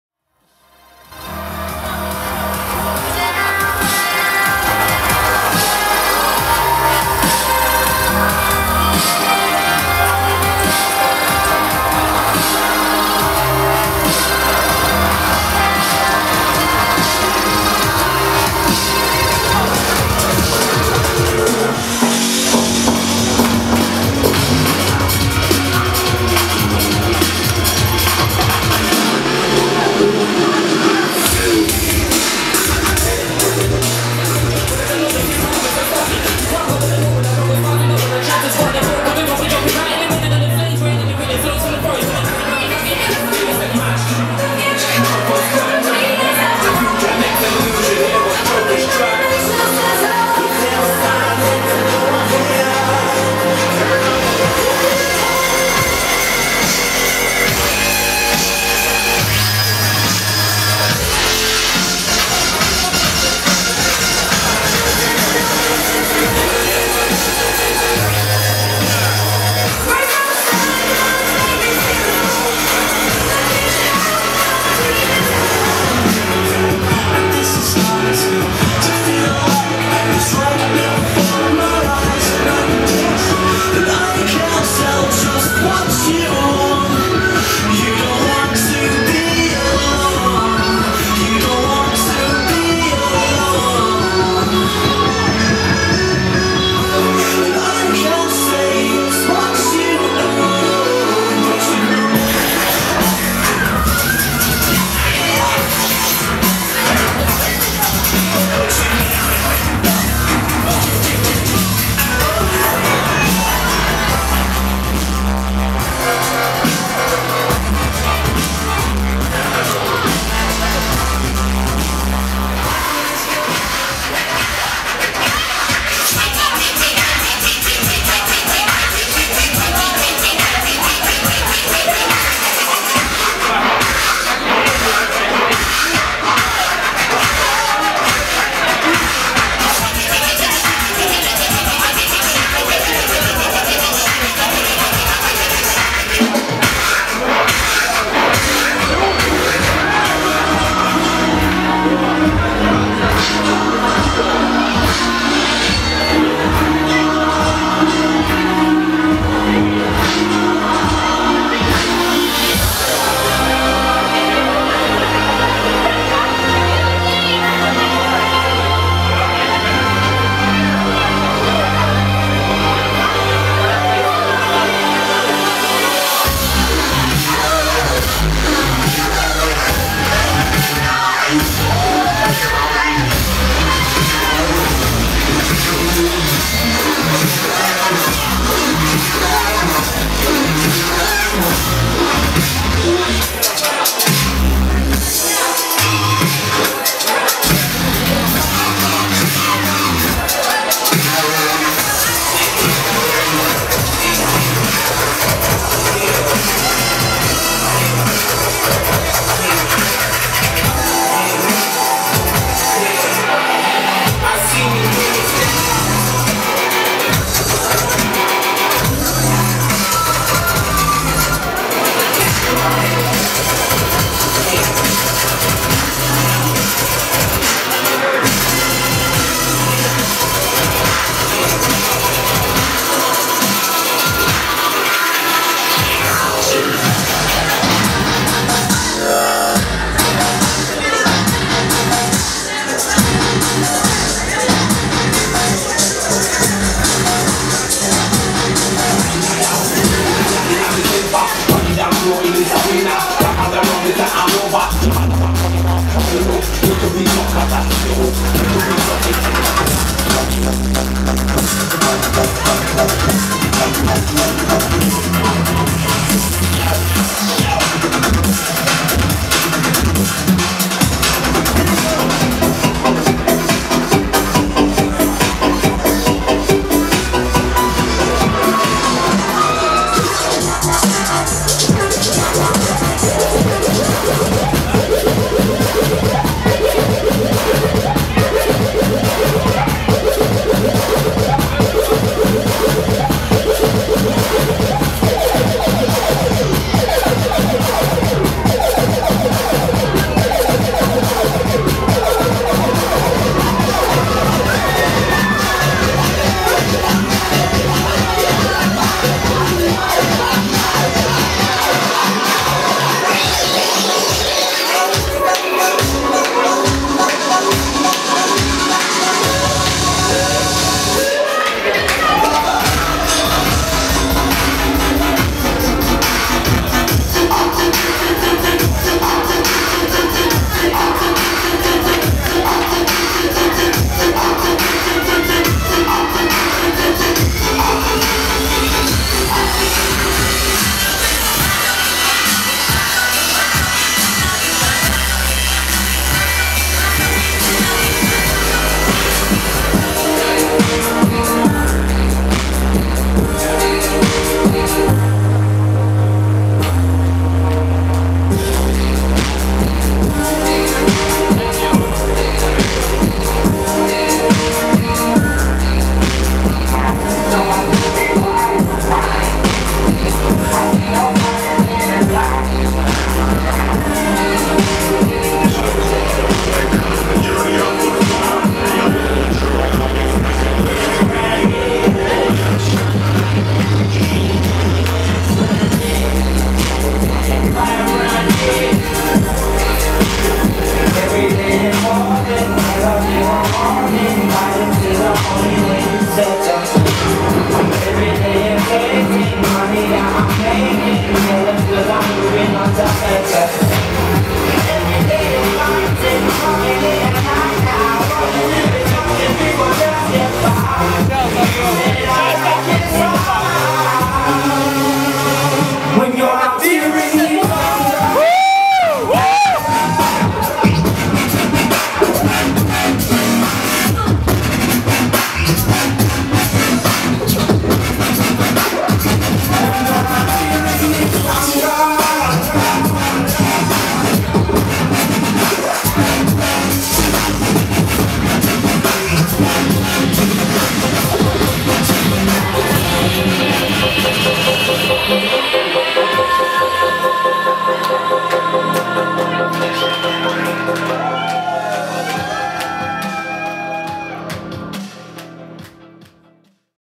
Bass Meating @ Ledo (02/11/11)
Dubstep party @ bar Ledo (Mariupol, Ukraine)

DJz:
Pussycut, DDHD, Shamon_D
Guest: